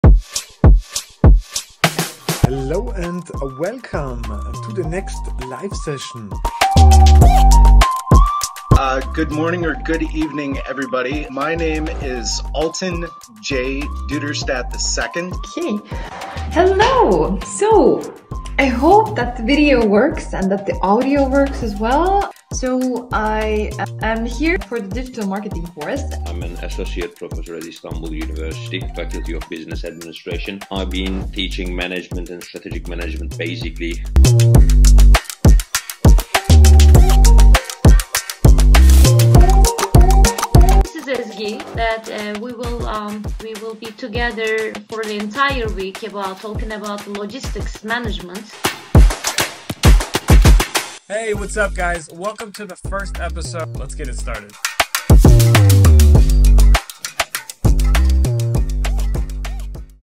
Free Online Courses with Certificates: Earn & Gain with IIENSTITU
Welcome to our free online courses!

Join educators like Markus Eden Hauser, Alton J. Duderstadt II, Liis Kambek, and many others as they guide you through topics like digital marketing, business management, and more.

Gain valuable knowledge and even earn certificates.